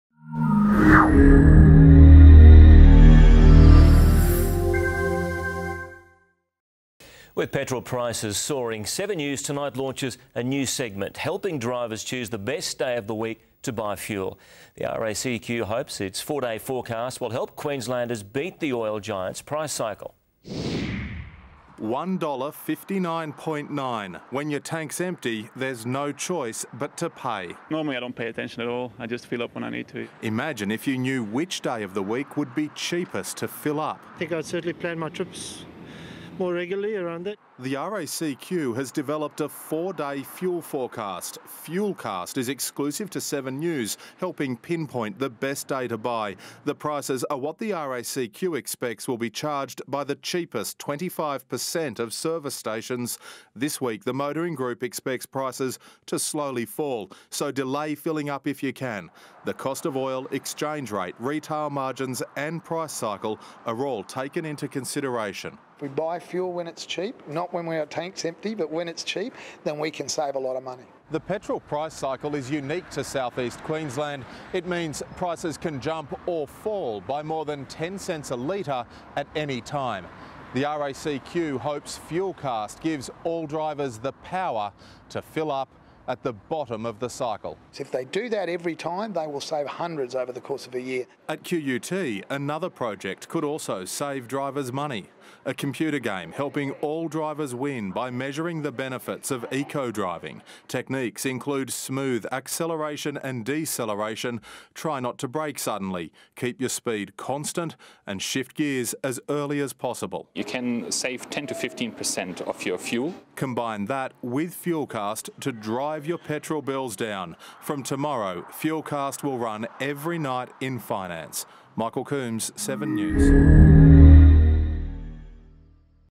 2013 Cube iCO2 eco racing game - Channel 7
QUT and Japan's National Institute of Informatics have launched an eco-friendly car racing game on the Cube - and the competition results will inform congestion-busting research projects.
In iCO2, the winner is not the first over the line but the person who travels the furthest on a tank of fuel.
QUT transport researchers will use data collected through the game to design traffic infrastructure geared for a population of eco-drivers.
Wide-spread eco-driving would cause major congestion on our existing road infrastructure because the traffic would take longer to pass through the road network.
But once researchers understand exactly how people behave as eco-drivers they can design new or adapt existing traffic infrastructure to cater for this new driving style.
Chanel 7's Brisbane news reported on the launch of iCO2 as part of a fuel-saving story on 15 July 2013.